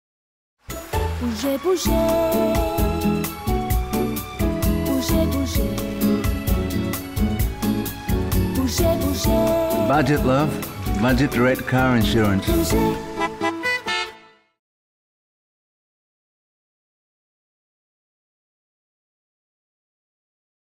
Budget Direct Car Insurance - Shower
Throughout 2009 and 2010 Budget Direct built on the success of the Michael and Michelle campaign by adding this now famous advert, which promoted our offer at that time, 'Pay Less or Get $50' in most Australian states. This 15 second television advertisement built on the previous adverts by adding a catchy new tune, delivered in Michelle's signature French style.